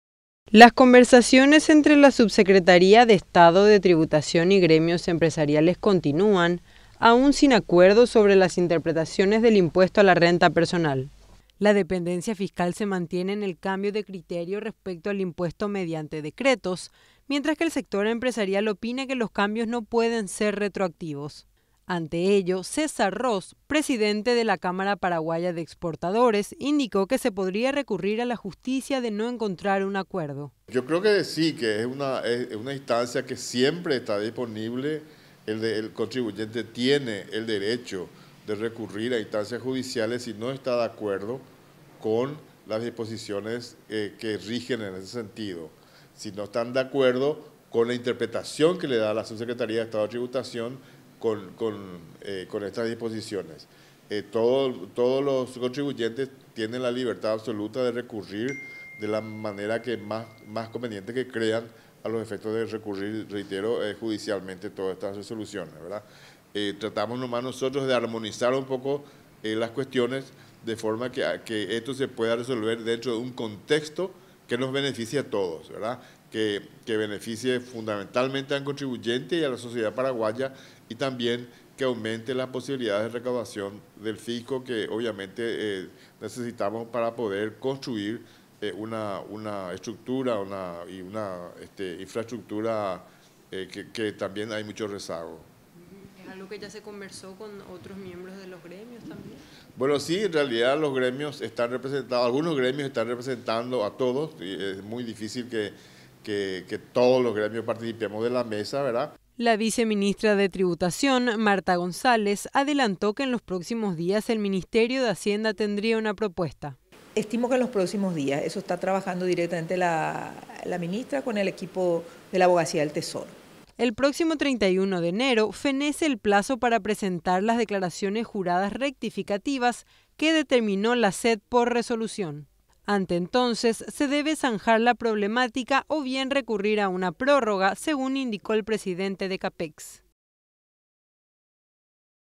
Gremios podrían apelar a la justicia por interpretaciones del IRP
Gremios empresariales podrían apelar a la justicia para aclarar las interpretaciones confusas del IRP indicó el presidente de la cámara de exportadores César Ros. La viceministra Marta González por su parte, adelantó que en los próximos días presentarán una nueva propuesta